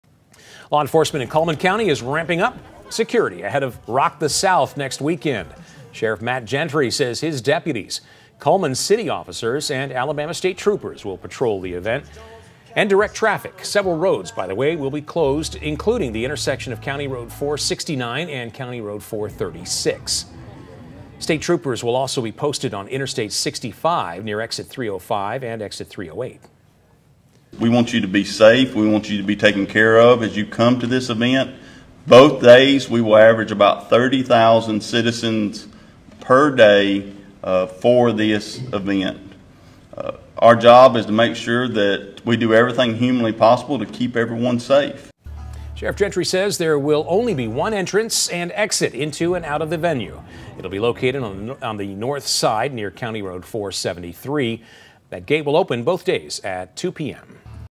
Staying safe at Rock The South in Cullman County
Increased security and police presence are just two of the ways officials hope to ensure a safe event this year